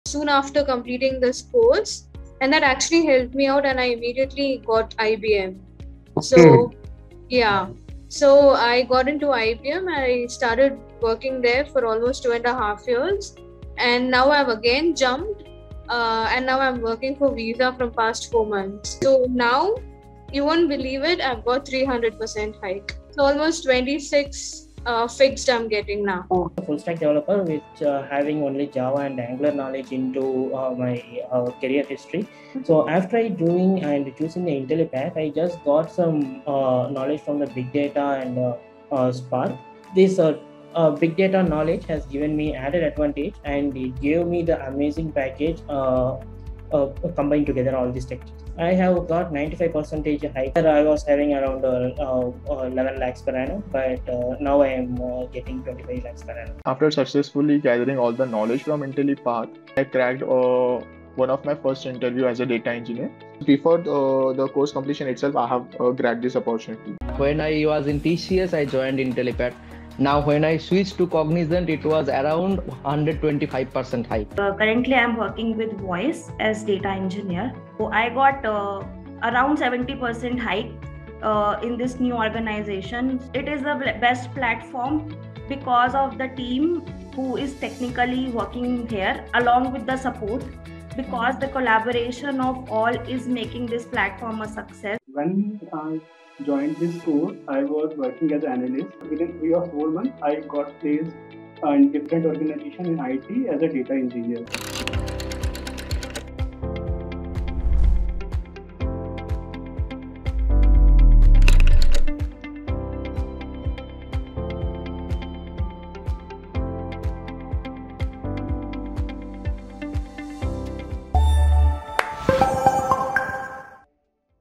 Best Data Engineering Course in India | Intellipaat Data Engineer Career Transition Reviews
Watch how Intellipaat Learners broke the mold and made groundbreaking career advancements after learning Intellipaat's Data Engineering Program.